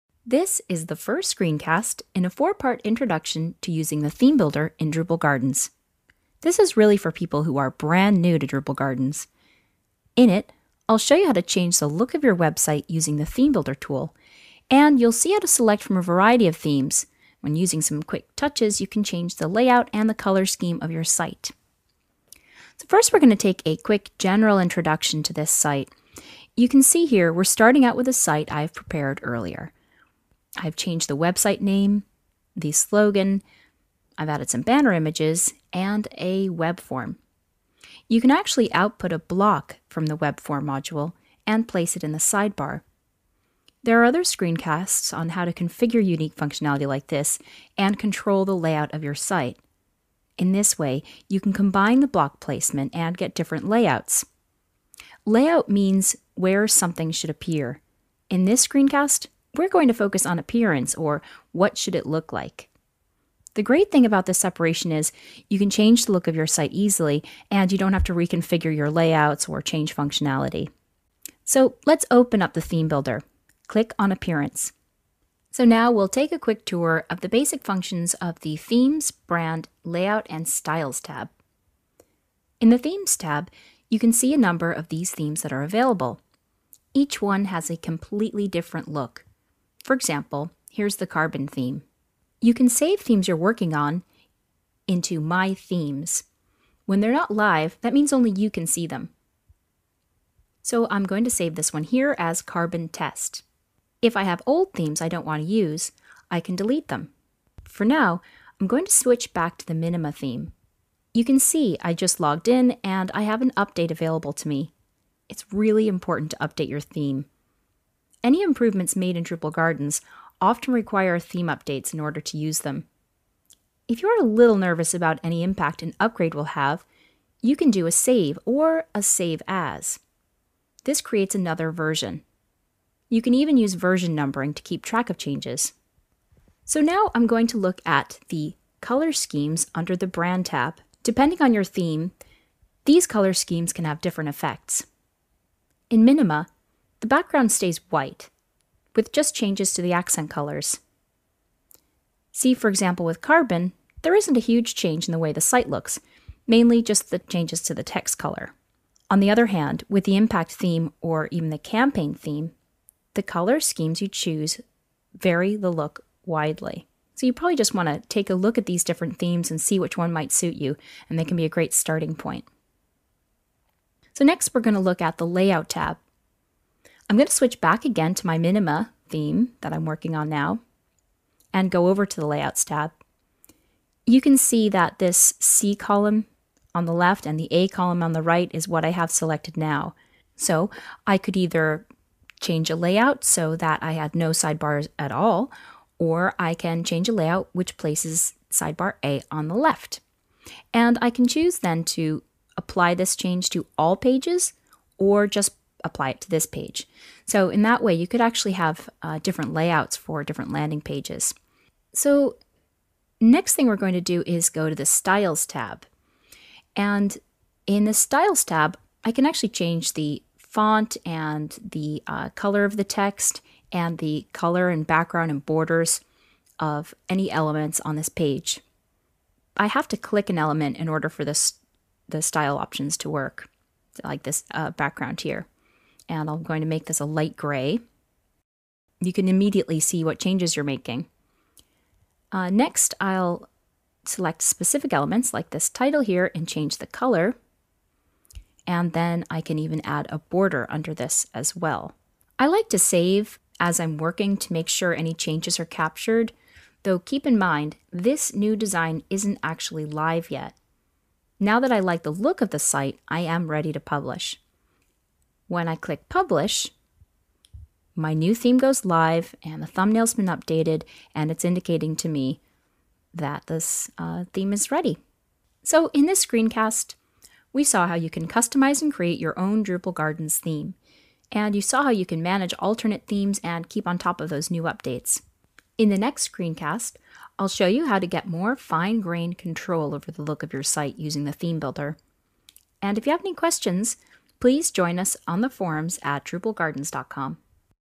Appearance - ThemeBuilder
This is the first screencast in a four-part introduction to using the ThemeBuilder in Drupal Gardens.
This  is for people who are new to Drupal Gardens. In it, I'll show you how to change the look of your website with the ThemeBuilder tool. You'll see how to select from a variety of themes, and with some quick touches, you can change the layout and color scheme of your site.